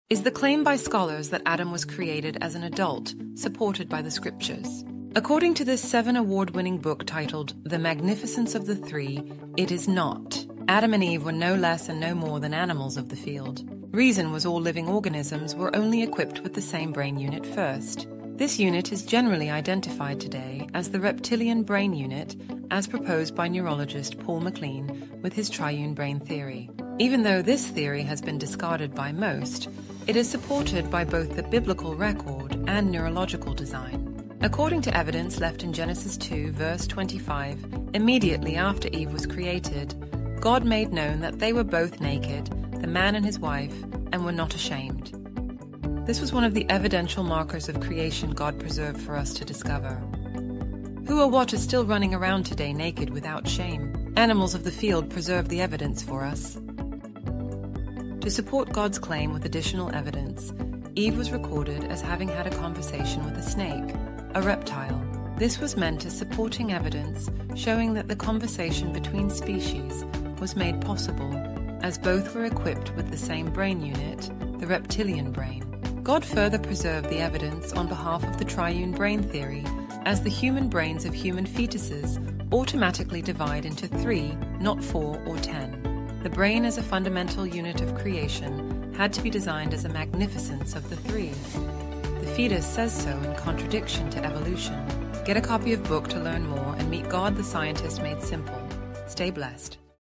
Adam and Eve with Reptilian Brain
Hello. Adam and Eve started out "naked and without shame" just like animals of the field then and still today. Neurological studies now explain the meaning of God's record with the Triune Brain theory.